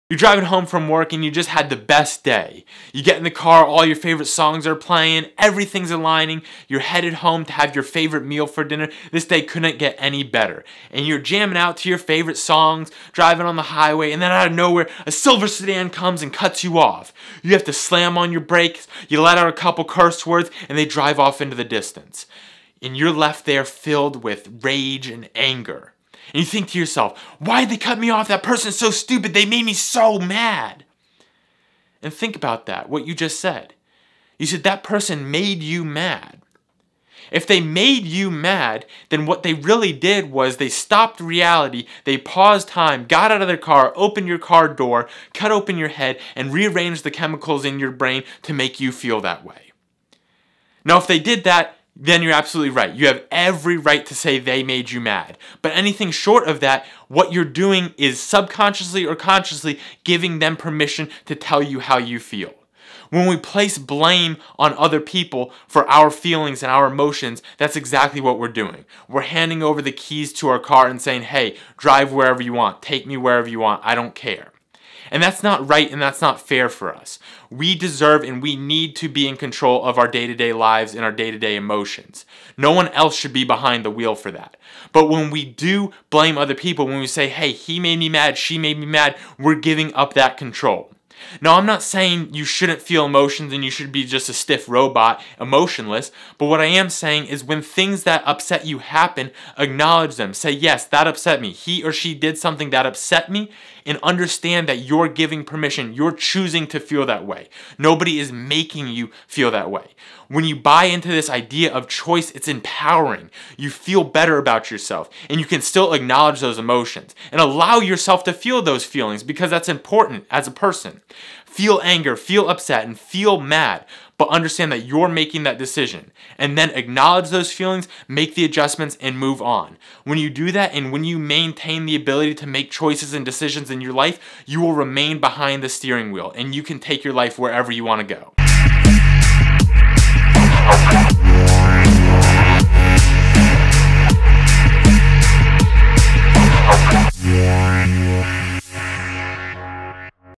Taking back control of your emotions
We may not feel like it all of the time, but we are always in control of our emotions.  When someone says or does something that upsets us or angers us, we choose to feel that way.  This isn't a bad thing, and it is not something we should ever avoid, but it is essential to understand that no one has the power to make you feel a certain way.

Acknowledge your emotions, allow yourself to feel them, make the necessary adjustments to get back on track, and move on.  You're in the driver seat; no one else should have permission to get behind the wheel.

This is a helpful piece of perspective on how to control your emotions.

New Book:
Flipping The Switch: How to Redefine Success to Find True Fulfillment